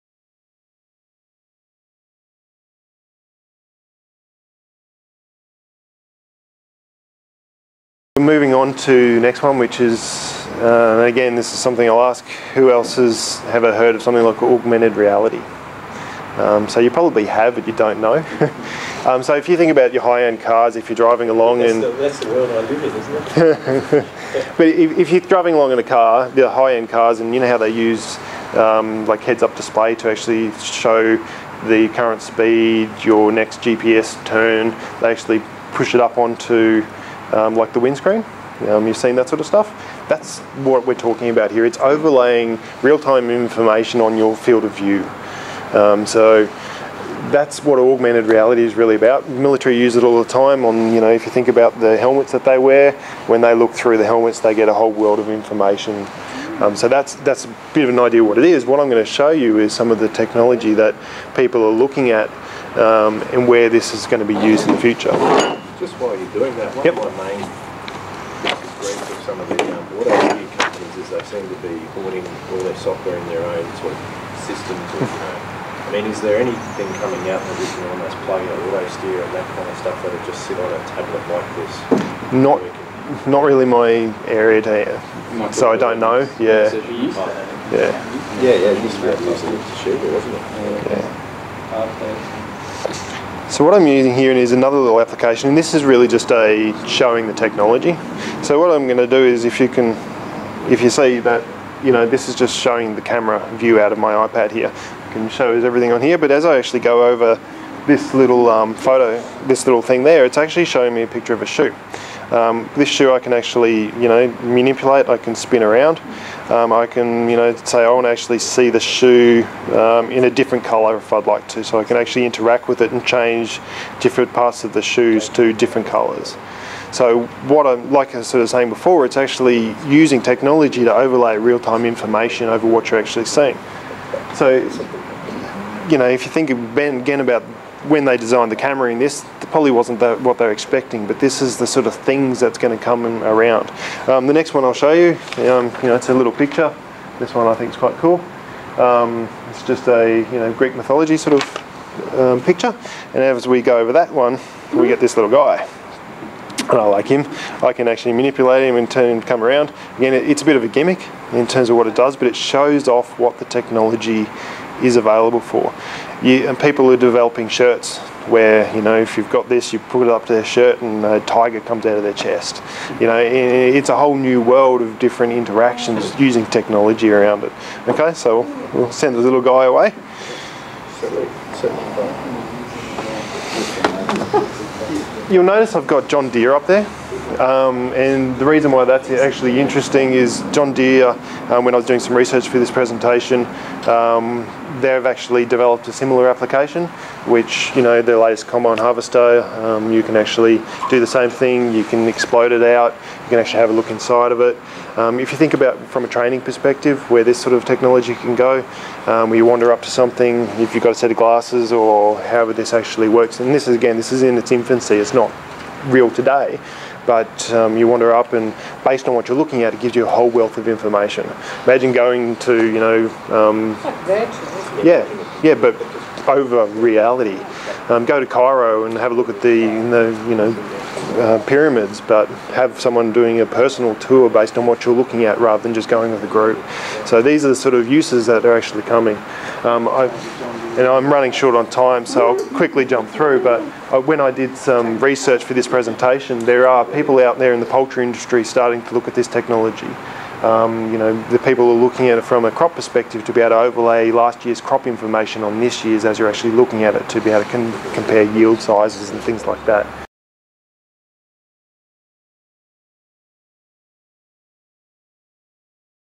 AgTech - Augmented reality
The right tools for the job - technology & agriculture

Presentation by John Henderson, Systems Engineer: DownsMicrosystems

Downs MicroSystems is the leading information and telecommunications solution provider in regional Southern Queensland. Ian and John will talk about some of the cool tools that are available and some appropriate technology solutions for your farming businesses.

AgTech Information Days - Helping take your farming business into the future.

Hosted by Condamine Alliance in Toowoomba and Dalby in June 2012 the day's connected people in the Darling Downs involved in the business of farming to professionals and technology to help make their businesses more effective and efficient -- from the office to the paddock.